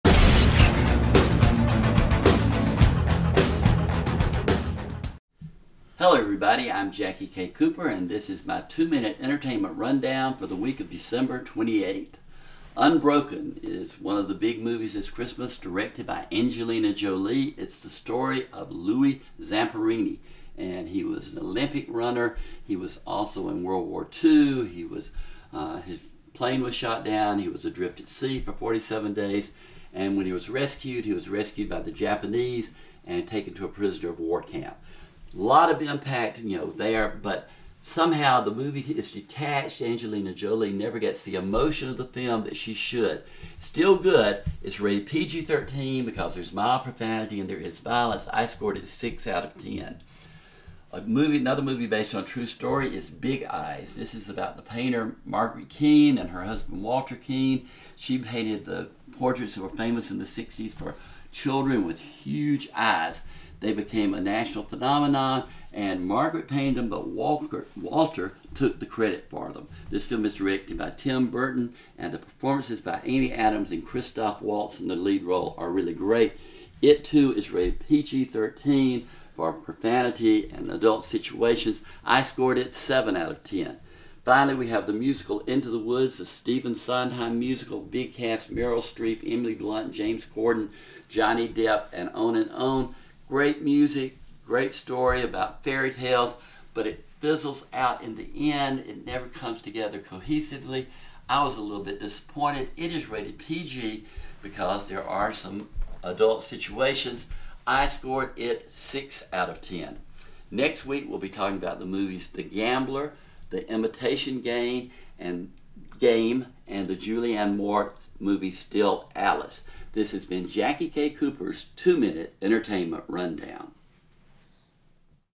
"Jackie K Cooper's Two Minute Entertainment Rundown" for the week of December 28, 2014.
Reviews of (1) Big Eyes (2) Into the Woods (3) Unbroken